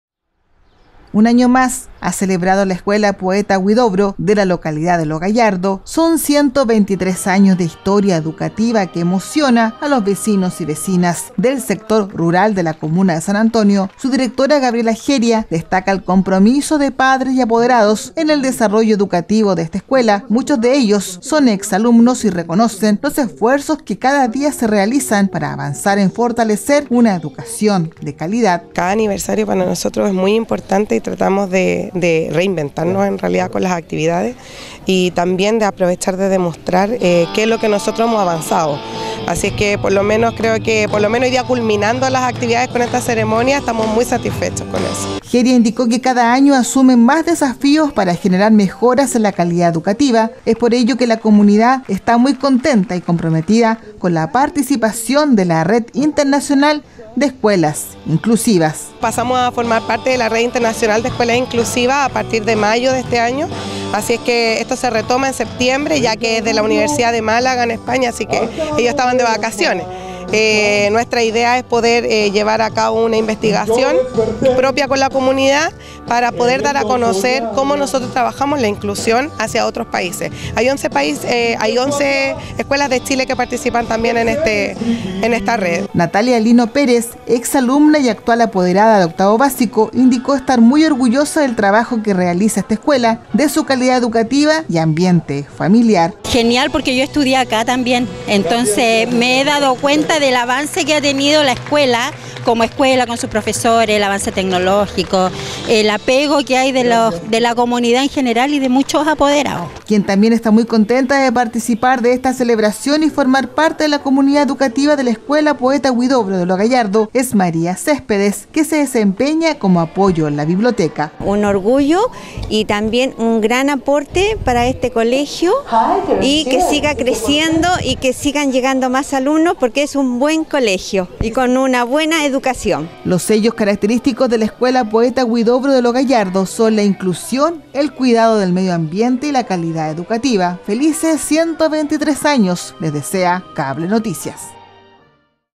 09 SEP 2024 Escuela Poeta Huidobro de Lo Gallardo celebra sus 123 años
Con diversos actos artísticos, coronación de reinas y un ambiente familiar, la comunidad educativa de la Escuela Poeta Huidobro celebra sus 123 años de historia. Una historia que une a varias generaciones y asume grandes desafíos para avanzar en el desarrollo escolar de la comuna de San Antonio.